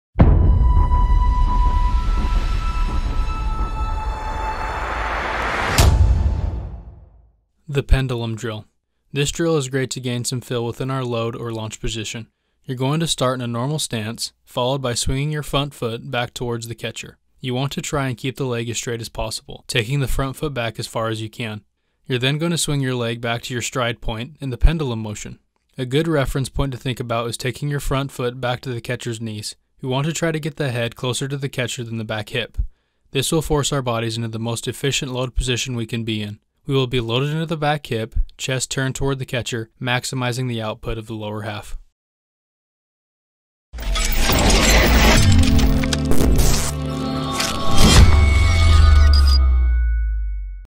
Silverback Sports Hitting - Pendulum Drill
The Pendulum Drill is great to gain some feel within our load or launch position. You’re going to start in a normal stance followed by swinging your front foot back towards the catcher. You want to try and keep the leg as straight as possible, taking the front foot back as far as you can. You’re then going to swing your leg back to your stride point in the pendulum motion. A good reference point to think about is taking your front foot back to the catcher’s knees. We want to try to get the head closer to the catcher than the back hip. This will force our bodies into the most efficient load position we can be in. We will be loaded into the back hip, chest turned toward the catcher, maximizing the output of our lower half.

Visit shopsilverback.com to view our entire line of the highest quality, most dependable baseball & softball training tools on the market.